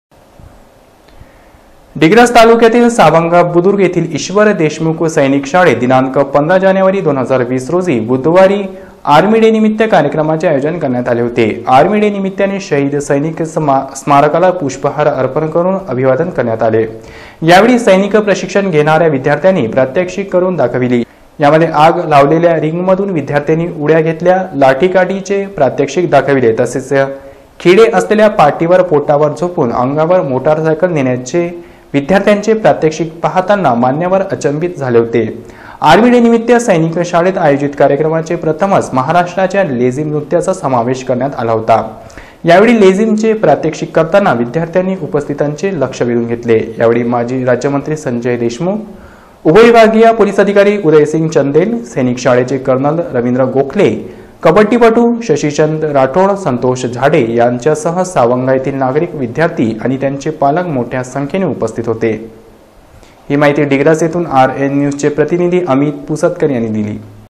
RN NEWS दिग्रस येथील सैनिक शाळेत आर्मी डे उत्साहात साजरा.
दिग्रस येथील सैनिक शाळेत आर्मी डे उत्साहात साजरा.